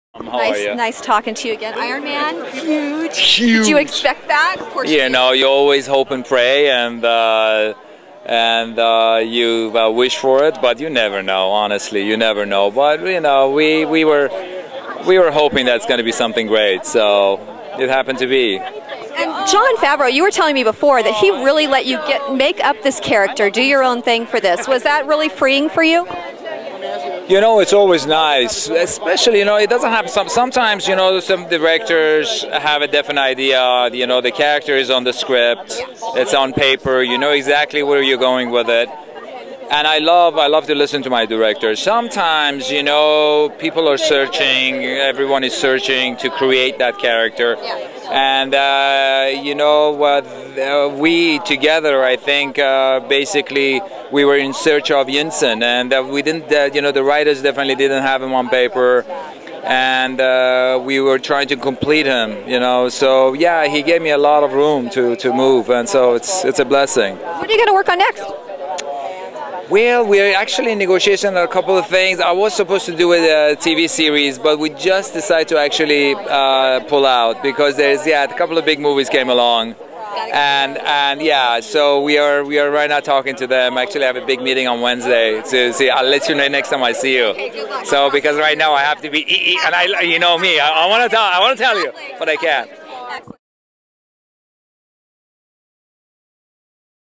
Shaun Toub Interview - 'Iron Man'
Interview with Shaun Toub who plays Yinsen in 'Iron Man.' The interview took place on the red carpet at the  2008 Los Angeles premiere of 'The Incredible Hulk.'

(Interviews are being consolidated and moved to YouTube which is why this went up in 2019 when it took place in 2008.)